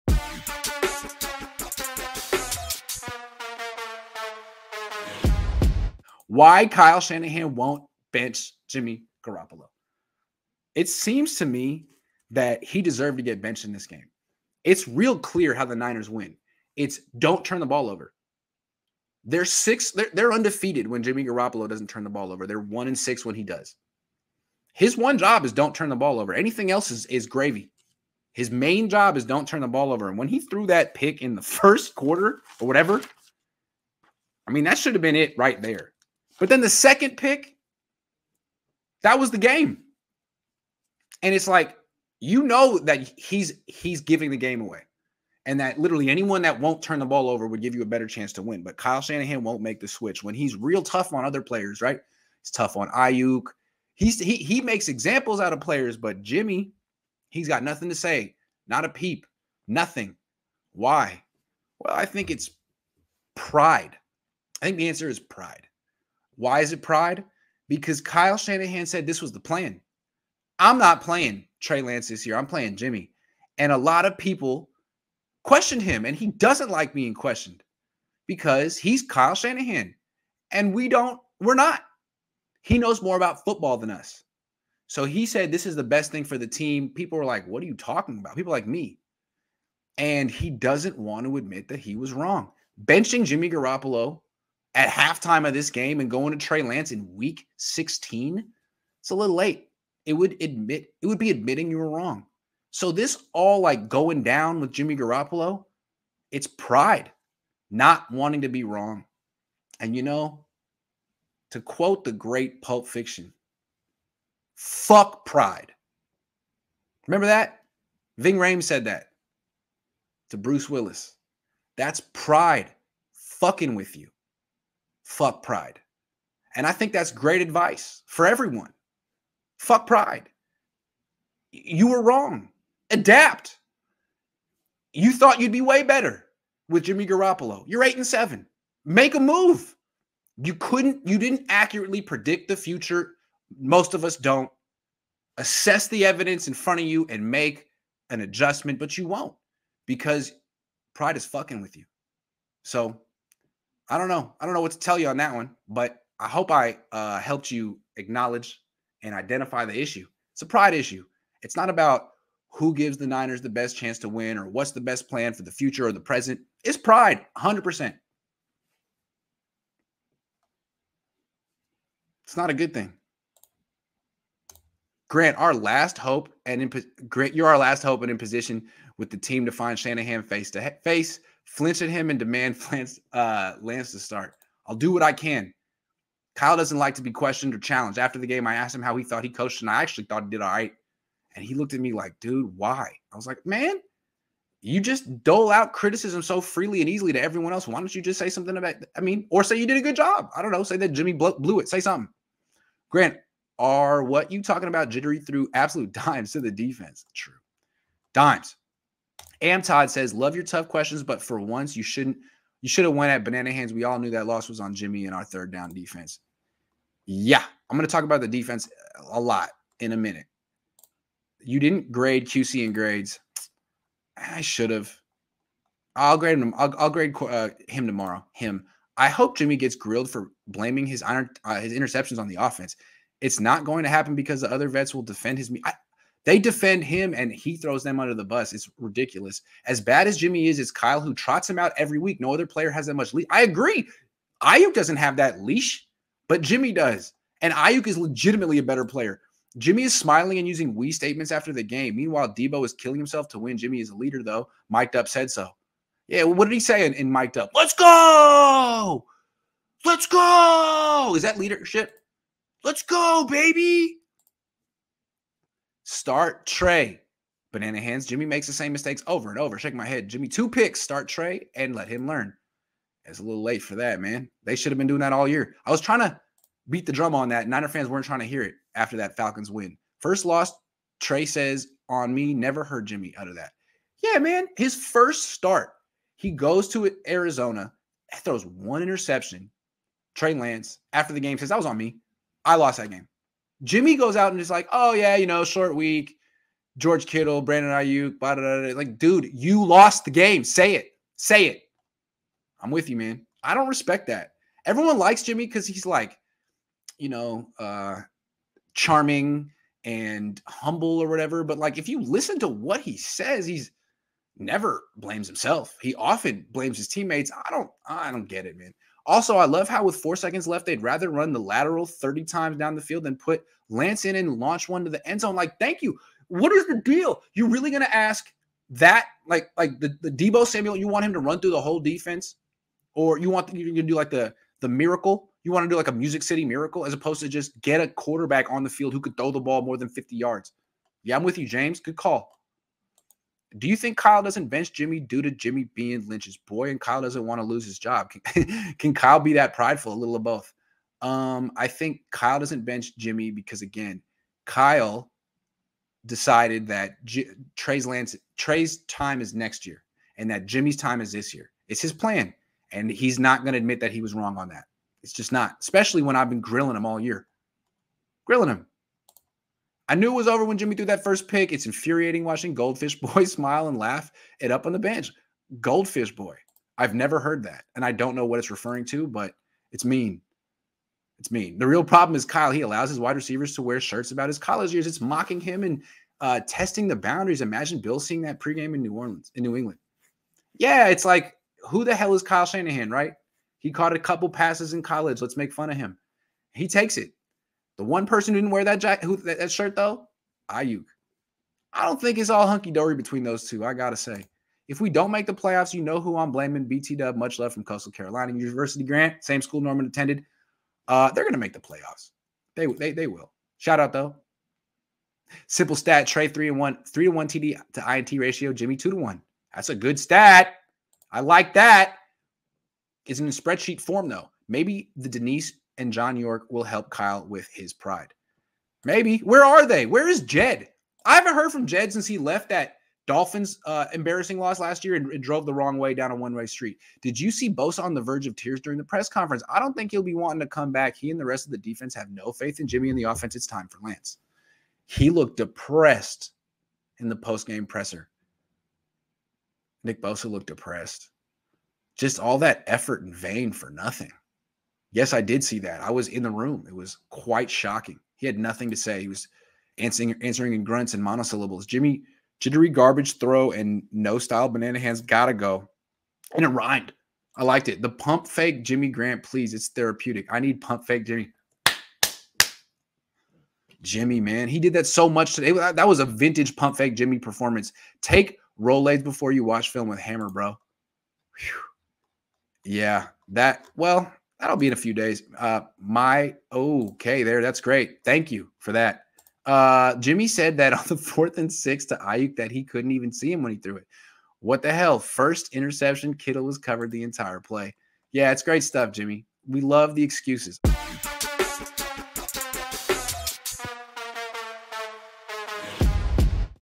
The Real Reason Kyle Shanahan Won’t Bench 49ers QB Jimmy Garoppolo
Grant Cohn discusses the real reason Kyle Shanahan won't bench San Francisco 49ers quarterback Jimmy Garoppolo.